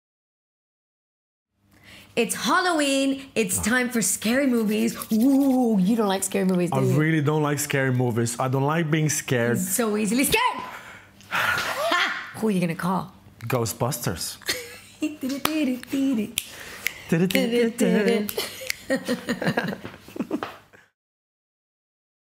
Aljaz & Janette love Halloween!
Two of our Strictly favourites Aljaz & Janette want to wish you a Happy Halloween! Join them for 'Remembering the Oscars' at The Hexagon on Thursday 21 April 2022.